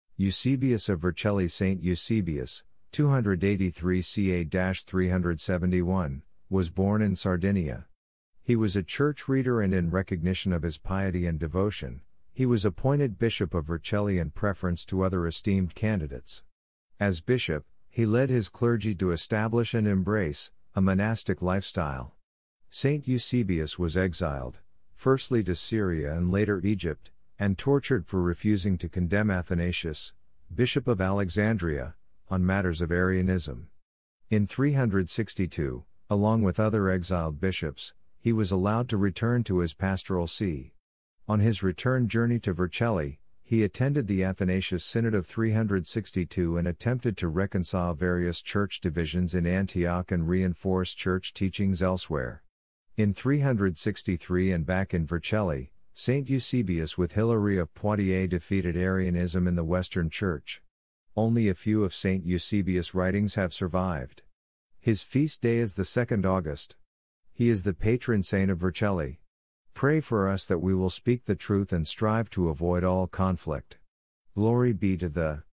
Eusebius of Vercelli
St Eusebius (283 ca – 371) was born in Sardinia. He was a Church Reader and in recognition of his piety and devotion, he was appointed Bishop of Vercelli in preference to other esteemed candidates. As Bishop, he led his clergy to establish and embrace, a monastic lifestyle.
 St Eusebius was exiled, firstly to Syria and later Egypt, and tortured for refusing to condemn Athanasius, Bishop of Alexandria, on matters of Arianism. In 362, along with other exiled Bishops, he was allowed to return to his Pastoral See. On his return journey to Vercelli, he attended the Athanasius synod of 362 and attempted to reconcile various church divisions in Antioch and reinforce church teachings elsewhere.
 In 363 and back in Vercelli, St Eusebius with Hilary of Poitiers defeated Arianism in the Western Church.
 Only a few of St Eusebius’ writings have survived. His Feast Day is the 2nd August. He is the Patron Saint of Vercelli.
 Pray for us that we will speak the truth and strive to avoid all conflict.
 Glory be to the…